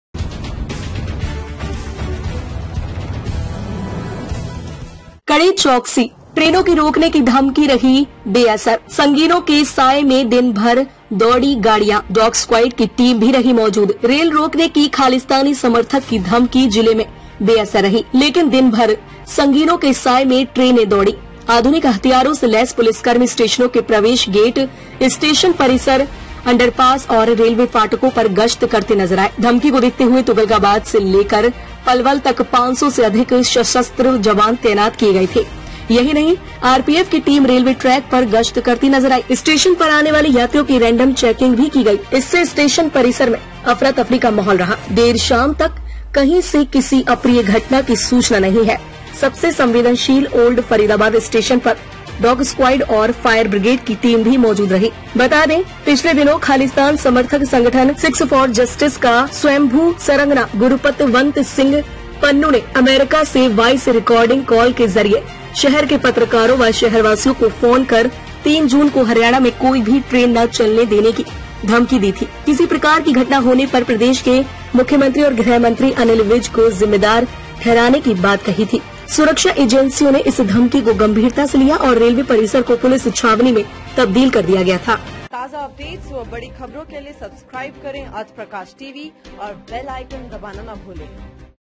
कड़ी चौकसी::ट्रेनों को रोकने की धमकी रही बेअसर, संगीनों के साए में दिन भर दौड़ी गाड़ियां
रेल रोकने की खालिस्तानी समर्थक की धमकी जिले में बेअसर रही। लेकिन दिन भर संगीनों के साए में ट्रेनें दौड़ी। आधुनिक हथियारों से लैस पुलिसकर्मी स्टेशनों के प्रवेश गेट, स्टेशन परिसर, अंडरपास आैर रेलवे फाटकों पर गश्त करते नजर आए।

धमकी को देखते हुए तुगलकाबाद से लेकर पलवल तक 500 से अधिक सशस्त्र जवान तैनात किए गए थे। यही नहीं आरपीएफ की टीम रेलवे ट्रैक पर गश्त करती नजर आयी। स्टेशन पर आने वाले यात्रियों की रेंडम चेकिंग भी की गई। इससे स्टेशन परिसर में अफरा तफरी का माहौल रहा। देरशाम तक कहीं से किसी अप्रिय घटना की सूचना नहीं है। सबसे संवेदनशील ओल्ड फरीदाबाद स्टेशन पर डॉग स्क्वायड और फायरबिग्रेड की टीम भी मौजूद रही।
बता दें पिछले दिनों खालिस्तान समर्थक संगठन 'सिख्स फॉर जस्टिस' का स्वयंभू सरगना गुरपतवंत सिंह पन्नू ने अमेरिका से वाइस रिकार्डिंग कॉल के जरिए शहर के पत्रकारों व शहरवासियों को फोन कर तीन जून को हरियाणा में कोई भी ट्रेन न चलने देने की धमकी दी थी। किसी प्रकार की घटना होने पर प्रदेश के मुख्यमंत्री और गृहमंत्री अनिल विज को जिम्मेदार ठहराने की बात कही थी। सुरक्षा एजेंसियों ने इस धमकी को गंभीरता से लिया और रेलवे परिसर को पुलिस छावनी में तब्दील कर दिया गया था।BREAKINGNEWS HEADLINES FATAFATNEWS ARTHPRAKASHTV ARTHPRAKASHNEWS#